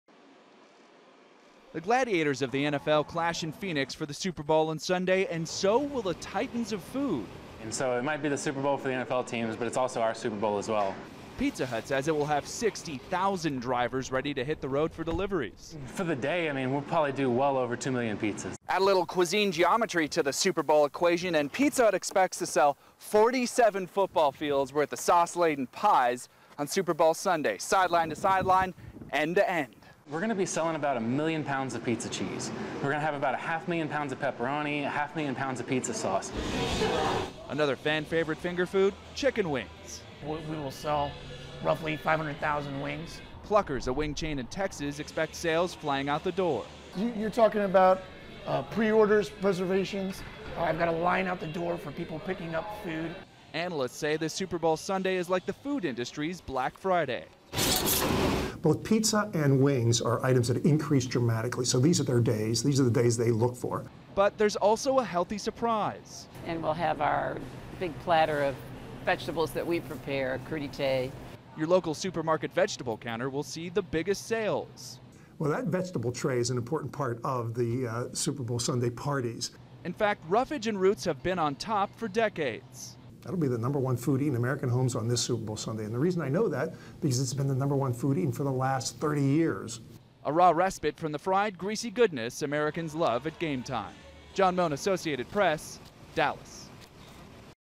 Fans Gear Up for Super Bowl of Snacks
America's football obsession reaches a crescendo this Super Bowl Sunday. But one winner already decided? The food. Analysts says it's the food industry's Black Friday. (Jan. 30)